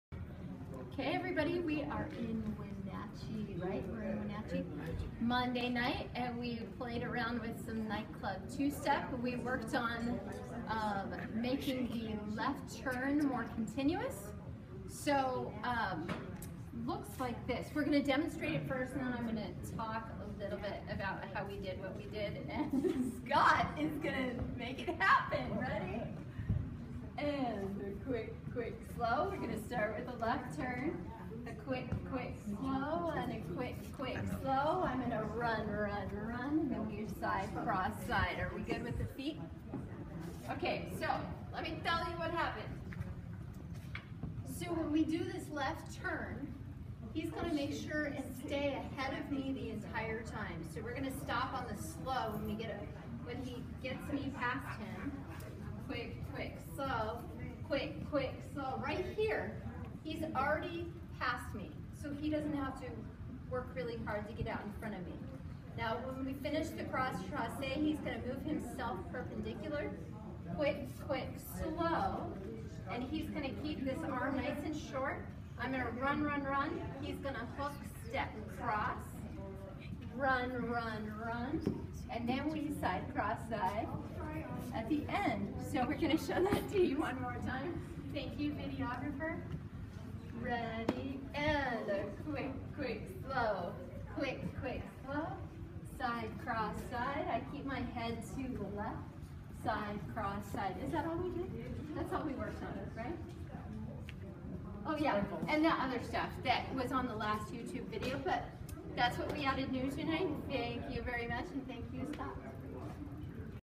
Nightclub 2-Step April 24, 2017
Monday Nights in Wenatchee, WA
Intermediate Nightclub 2-step

Left turn, Run-Run-Run, Cross Chasse